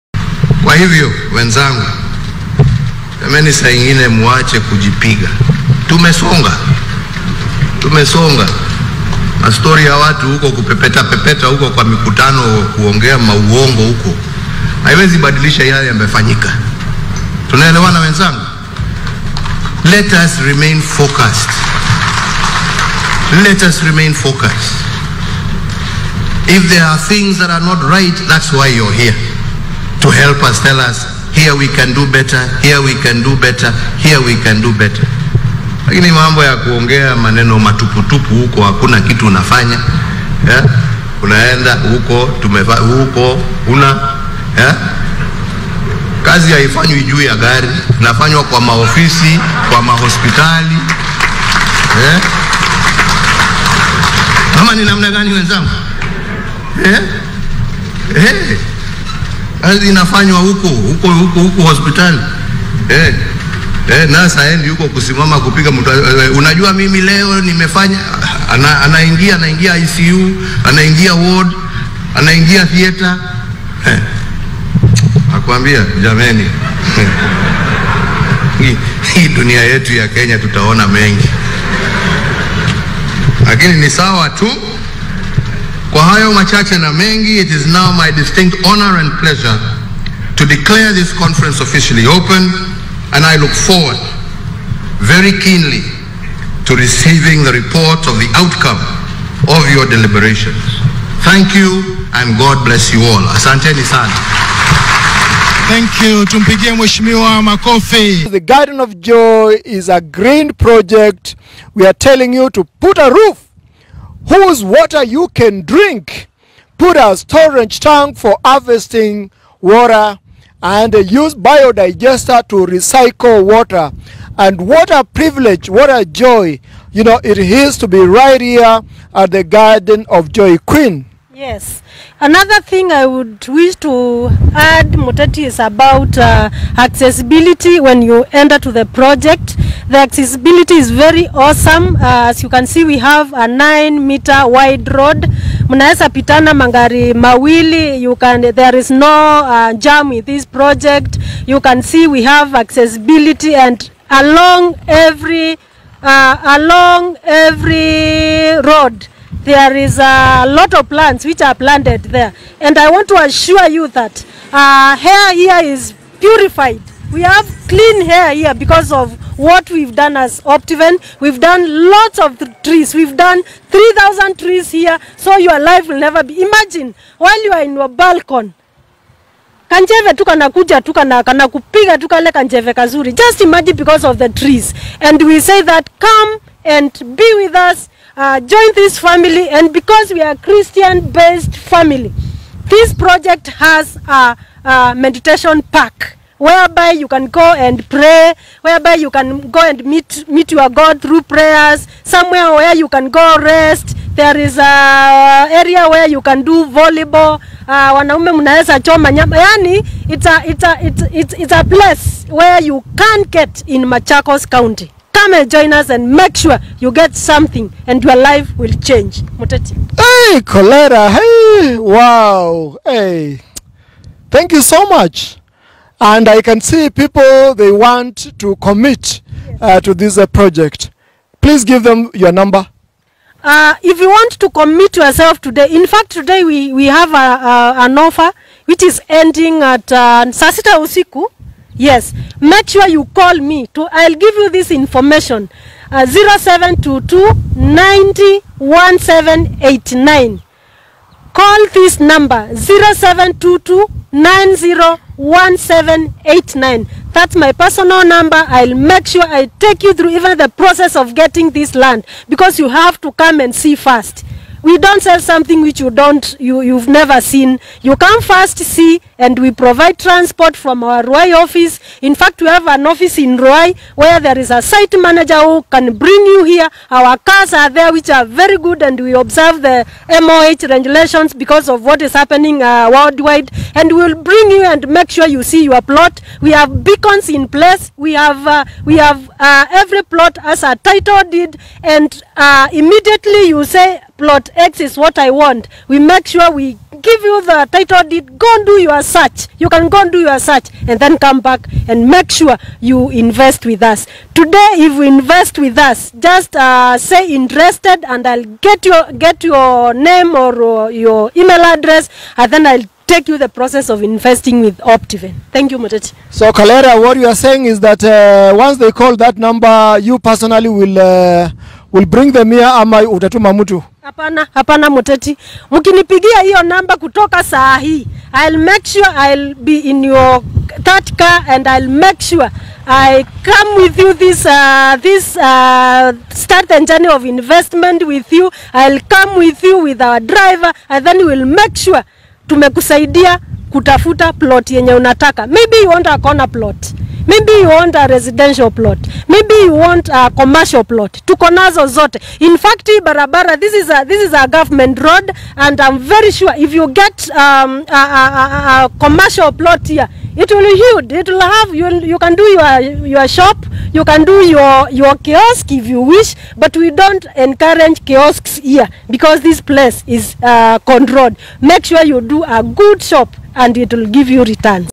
ANGRY UHURU KENYATTA WARNS DP RUTO || KAZI HAIFANYWI JUU YA MAGARI!!
UHURU TELLS RUTO OFF. HE REMINDED HIM THAT PEOPLE DONT WORK FROM CONVOYS OF VEHICLES.